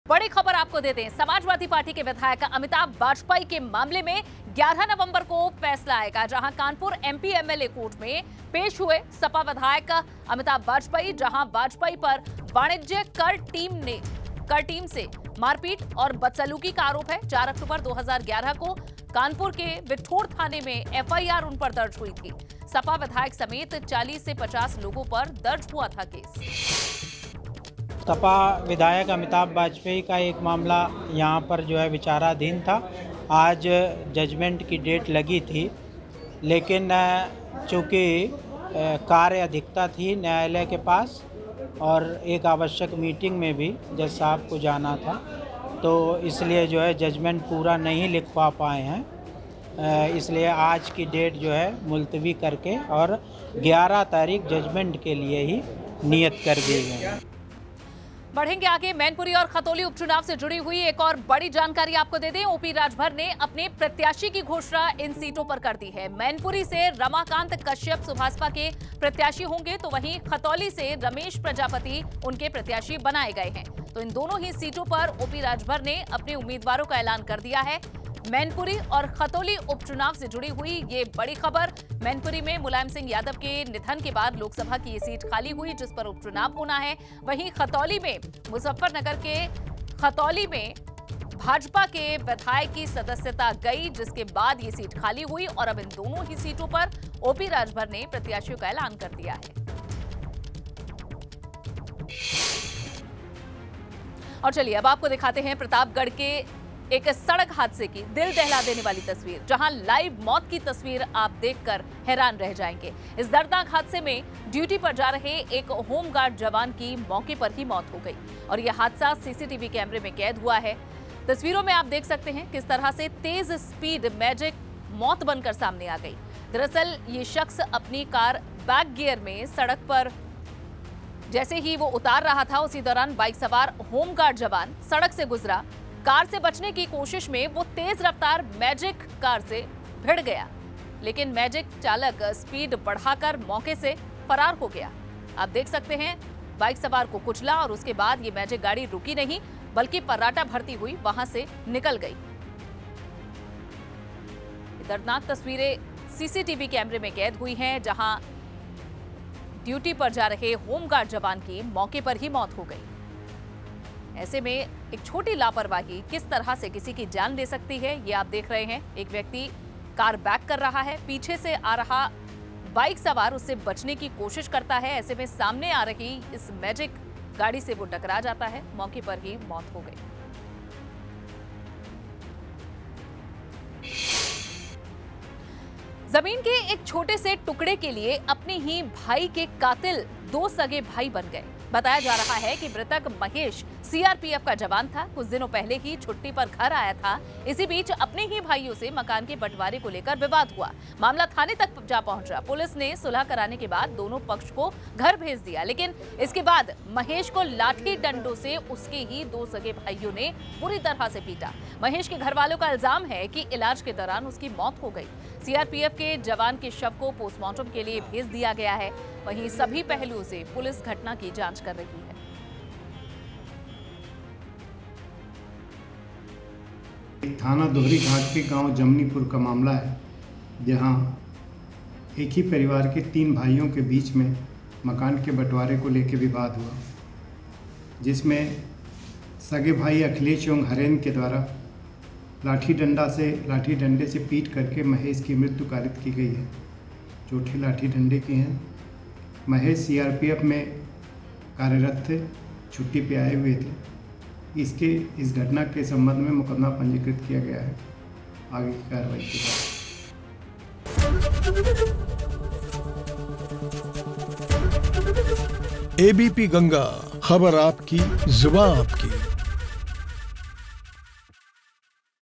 सपा विधायक अमिताभ वाजपेई कानपुर MP-MLA कोर्ट में पेश हुए, 11 नवंबर को फैसला
सपा विधायक अमिताभ वाजपेई के मामले में 11 नवंबर को फैसला
कानपुर MP-MLA कोर्ट में पेश हुए सपा विधायक अमिताभ वाजपेई
वाजयेई पर वाणिज्य कर टीम से मारपीट-बदसलूकी का आरोप
4 अक्टूबर 2011 को कानपुर के बिठूर थाने में दर्ज हुई थी FIR
सपा विधायक समेत 40-50 लोगों पर दर्ज हुआ था मुकदमा